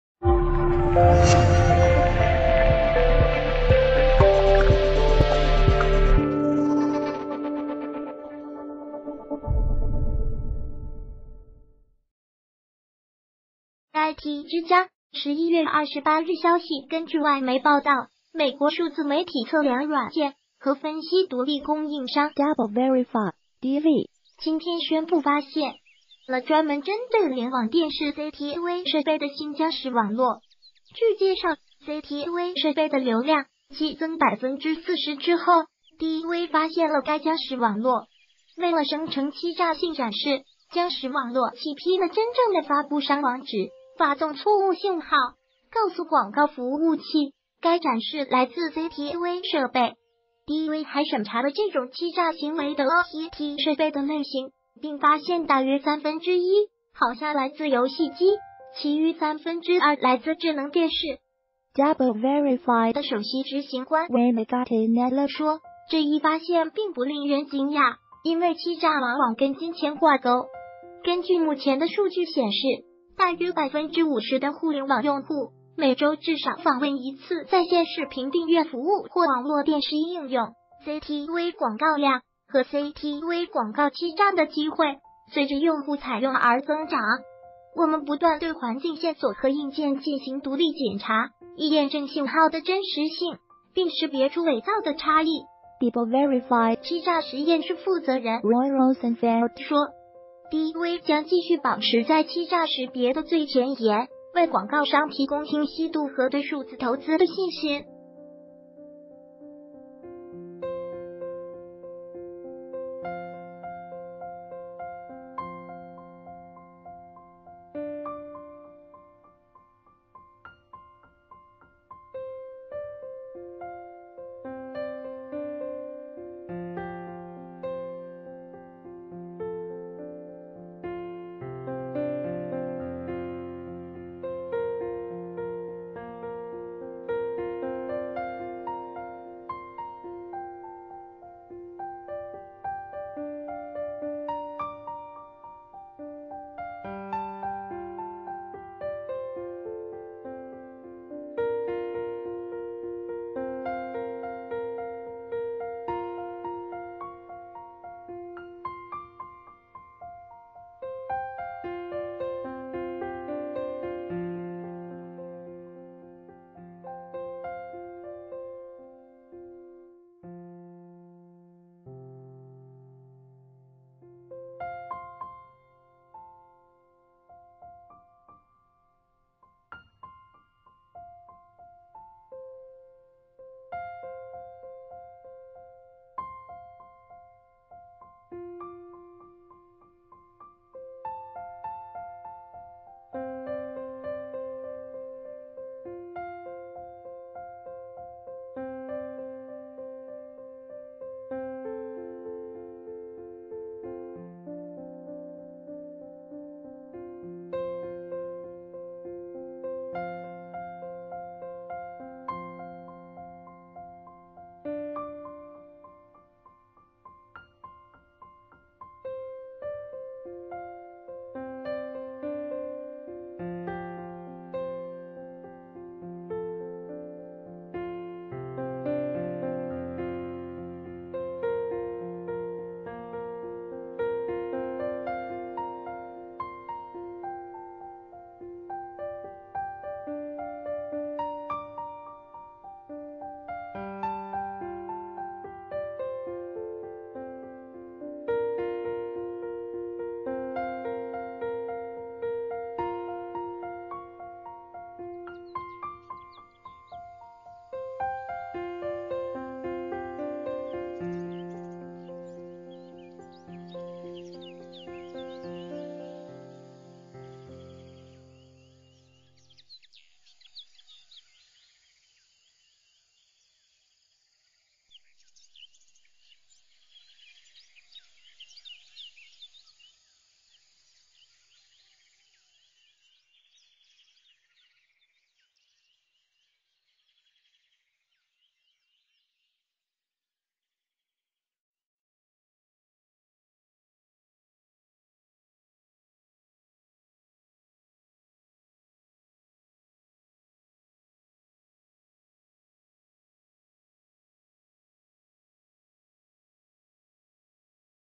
新型僵尸网络出现：专门攻击智能电视
谢谢！IT之家11月28日消息根据外媒报道，美国数字媒体测量软件和分析独立供应商DoubleVerify（“DV”）今天宣布发现了专门针对联网电视（CTV）设备的新僵尸网络。据介绍，CTV设备的流量激增40%之后，DV发现了该僵尸网络。为了生成欺诈性展示，僵尸网络欺骗了真正的发布商网址，发送错误信号，告诉广告服务器该展示来自CTV设备。DV还审查了这种欺诈行为的OTT设备的类型，并发现大约三分之一好像来自游戏机，其余三分之二来自智能电视。DoubleVerify的首席执行官WayneGattinella说：“这一发现并不令人惊讶，因为欺诈往往跟金钱挂钩。”“根据目前的数据显示，大约50%的互联网用户每周至少访问一次在线视频订阅服务或网络电视应用，CTV广告量和CTV广告欺诈的机会随着用户采用而增长。”“我们不断对环境线索和硬件进行独立检查，以验证信号的真实性，并识别出伪造的差异，”DoubleVerify欺诈实验室负责人RoyRosenfeld说。“DV将继续保持在欺诈识别的最前沿，为广告商提供清晰度和对数字投资的信心。”